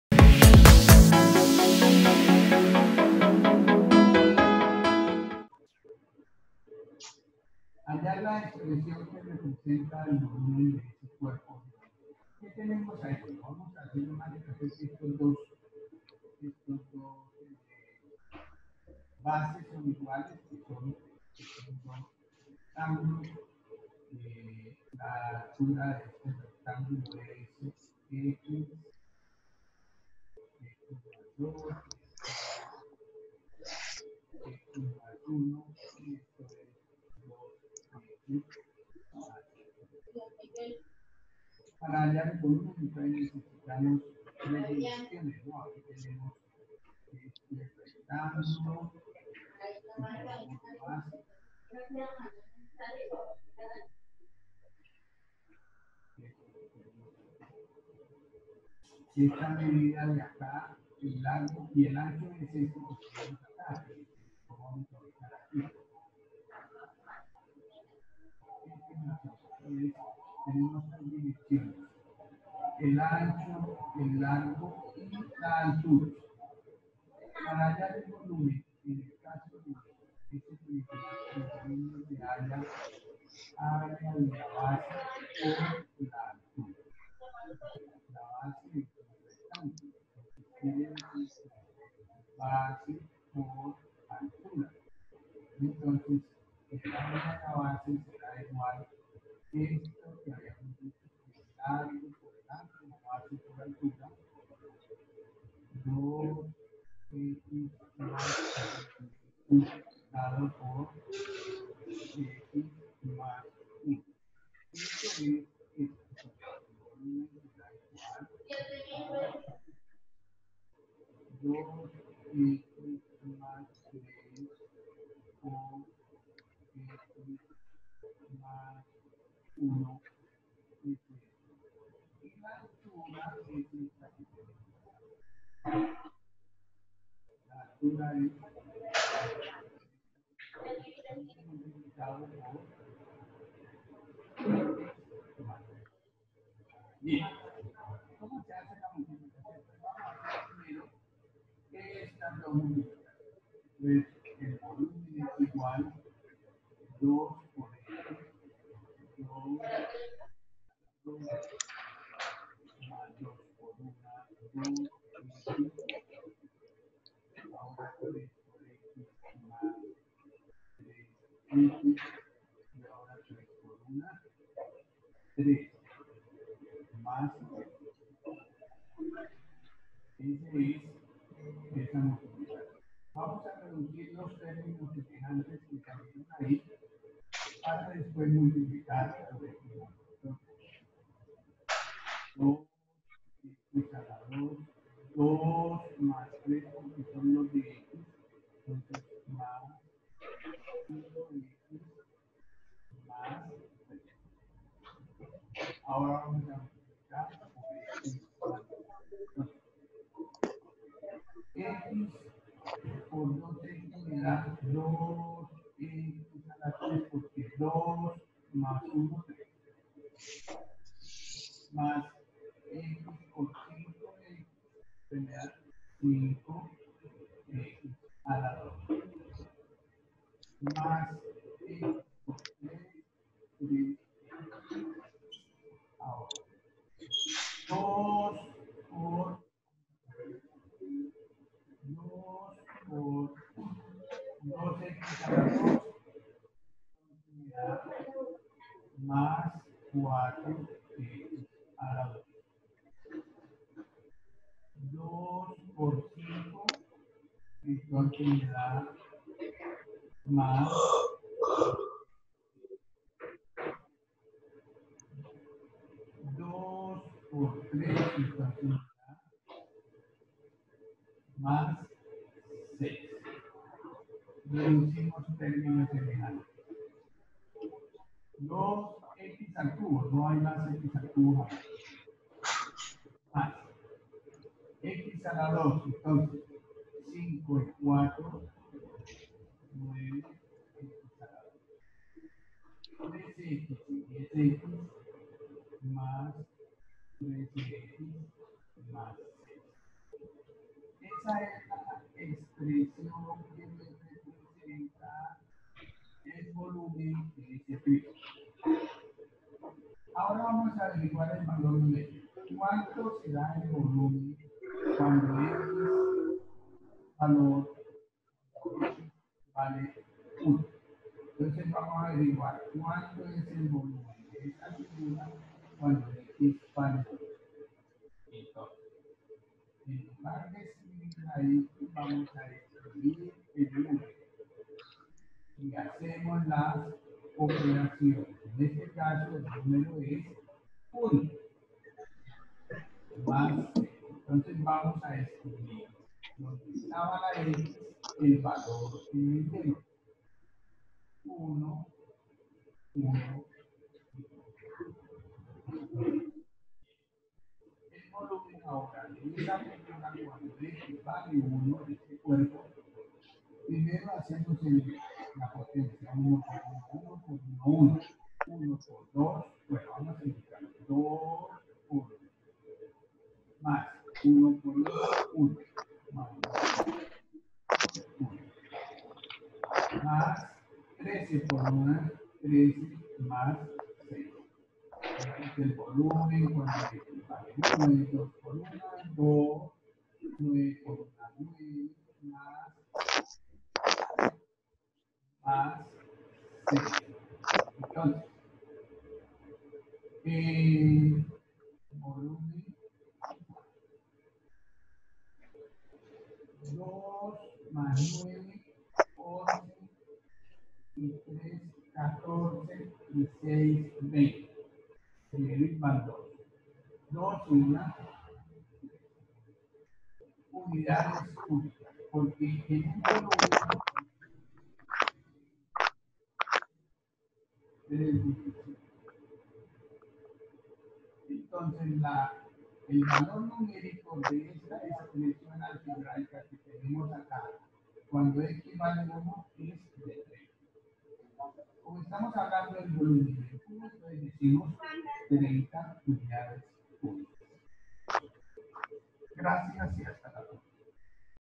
APLICACIONES DEL ÁLGEBRA VOLUMEN DE UN CUERPO GEOMÉTRICO
Hallar la expresión que representa el volumen de un prisma